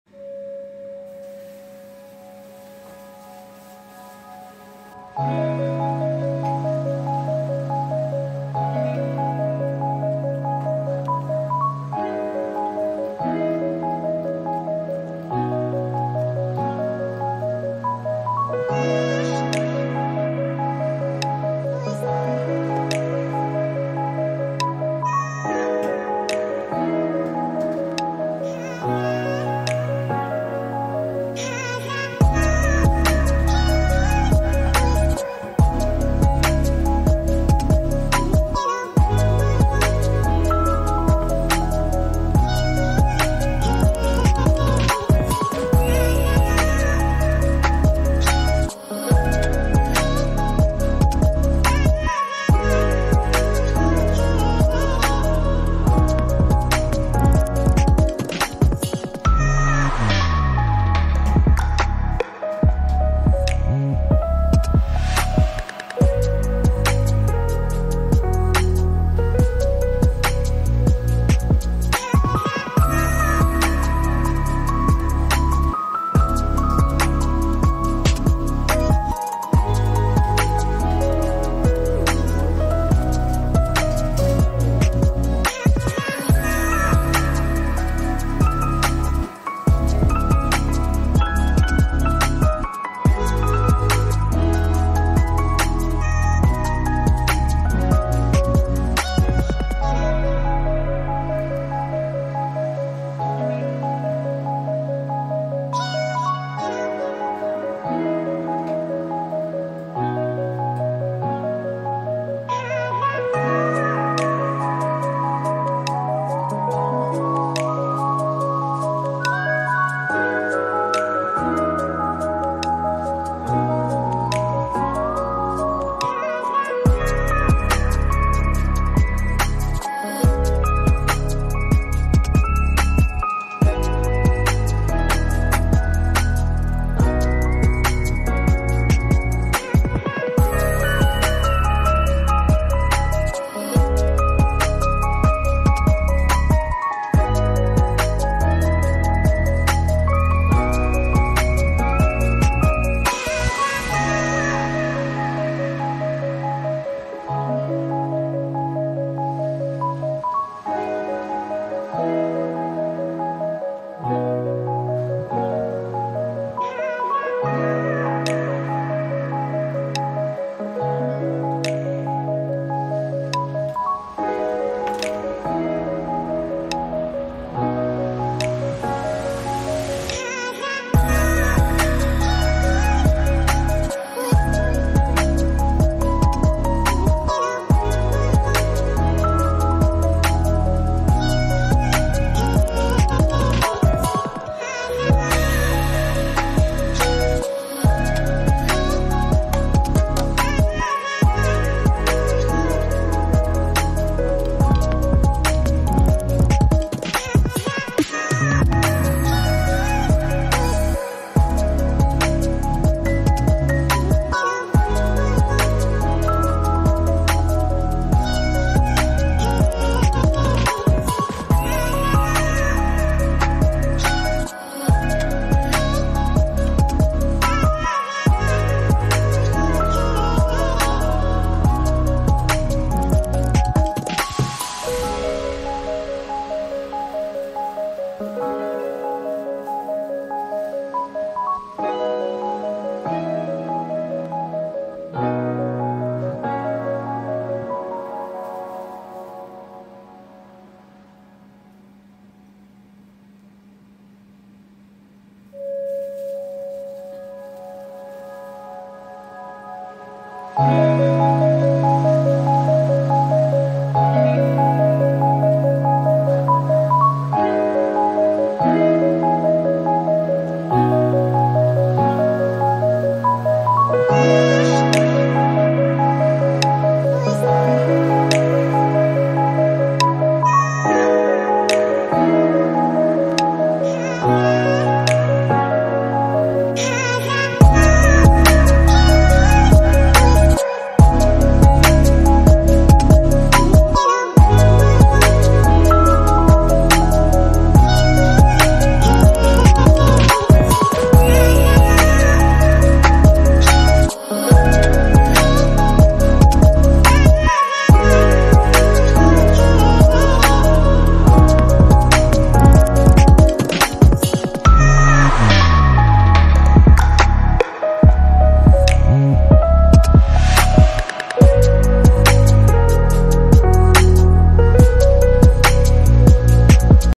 Super Easiest Method For Pizza
Hello guys, as promised, we make a type of pizza every week, but today I decided to make Cuban pizza in the easiest and fastest way.
Our pizza dough was already prepared and explained in the previous video
Pizza dough ingredients
ketchup
Cheese Pizza
It doesn't matter if there is no sausage, but it gives a better taste
180 degrees for fifteen minutes